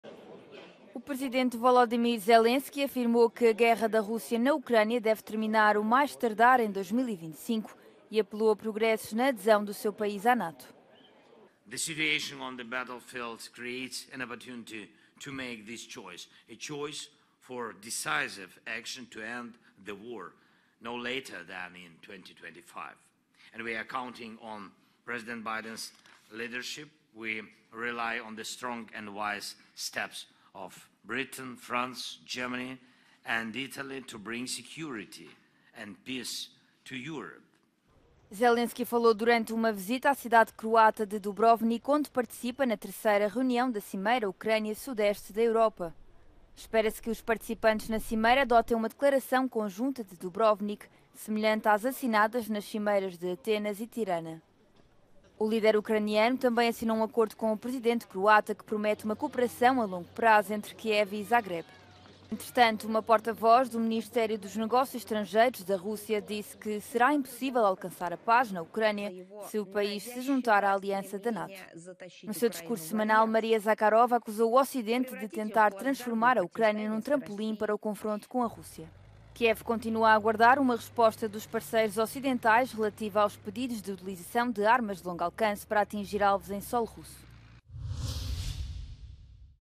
Presidente Zelenskyy: A guerra da Rússia na Ucrânia deve terminar o mais tardar em 2025
Zelenskyy falou durante uma visita à cidade croata de Dubrovnik, onde participa na terceira reunião da cimeira Ucrânia-Europa do Sudeste.